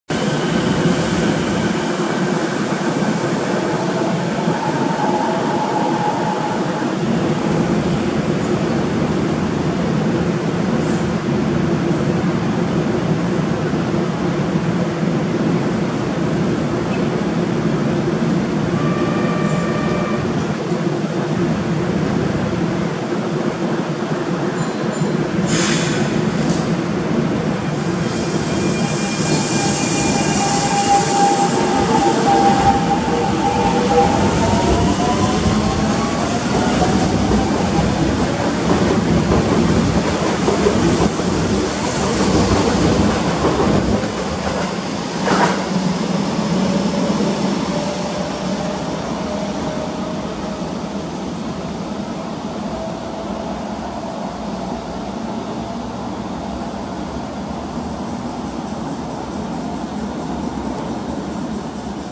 【3000形も置き換え対象...】 神戸市営地下鉄西神・山手線3000形3125F 妙法寺駅発車
神戸市営地下鉄西神・山手線3000形3125Fです。
1992年に登場した3000形も全編成6000形に置き換えられる予定です。1976年に登場した1000形と同時期に置き換えられることを考えると，3000形は短命になってしまいそうです。3000形は編成数が少ないので早めに撮影したいところです。

B4920
DSCN1393